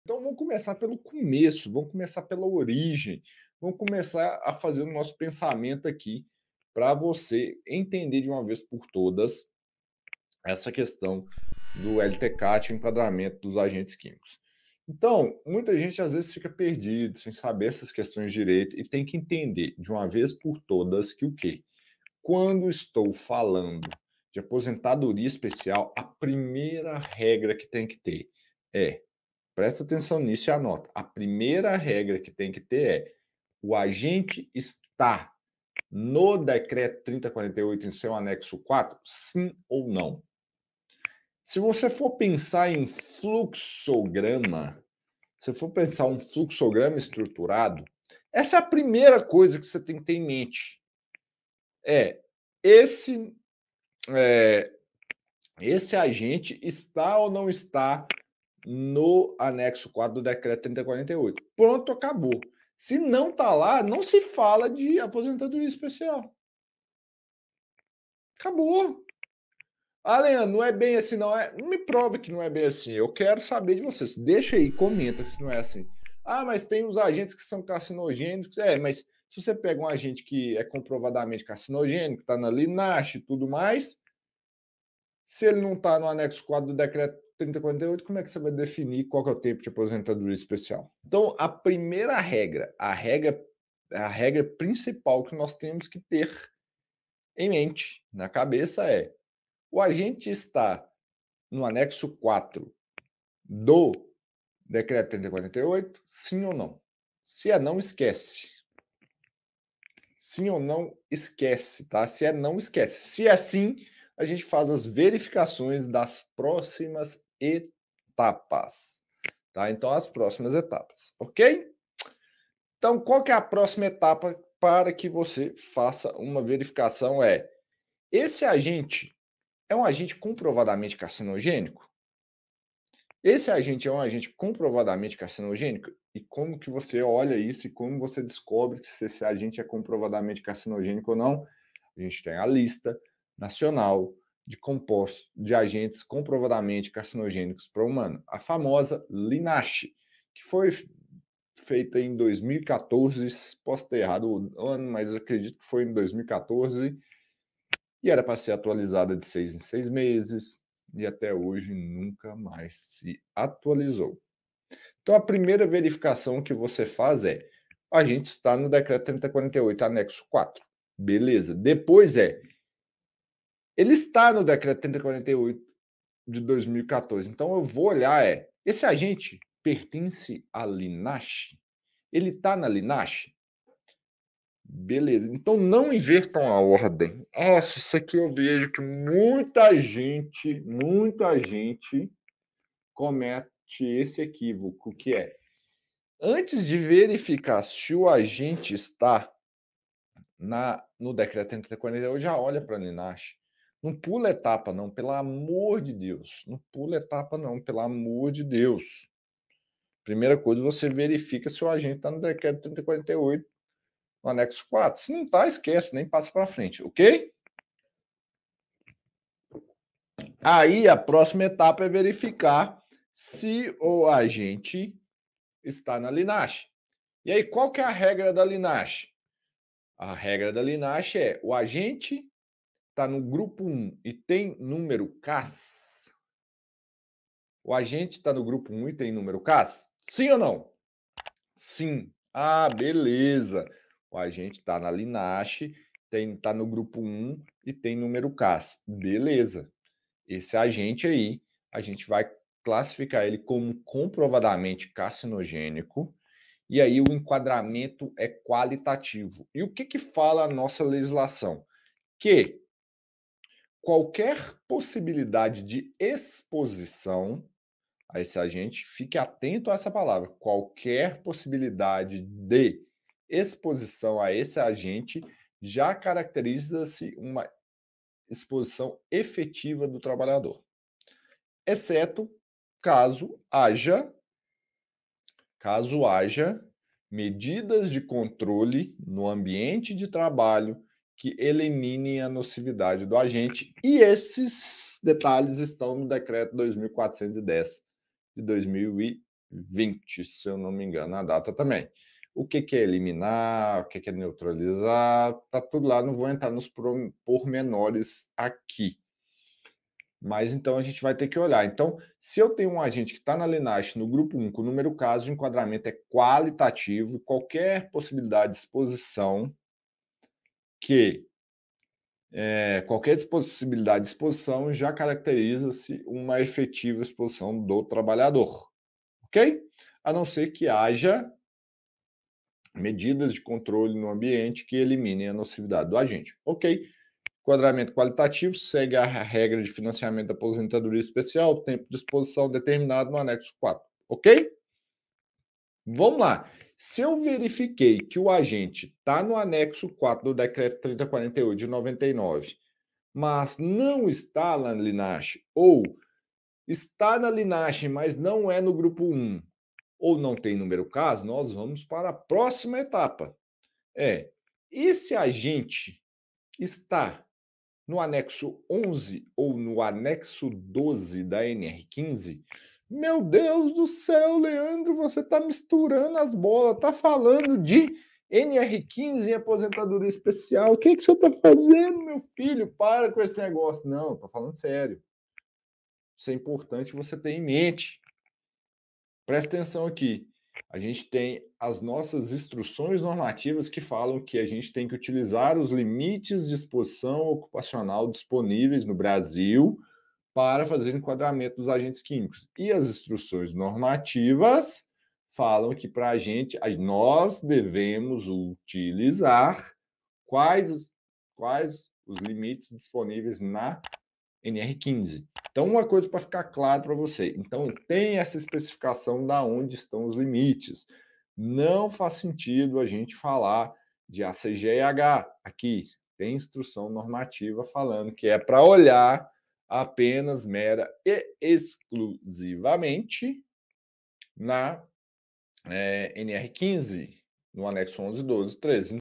Como enquadrar riscos químicos na aposentadoria especial? - Expresso HO #66
Neste vídeo, eu vou te mostrar tudo que você precisa saber e aplicar para fazer o enquadramento correto dos agentes químicos na legislação previdenciária para fazer os envios para o eSocial, acompanha tudo até o final.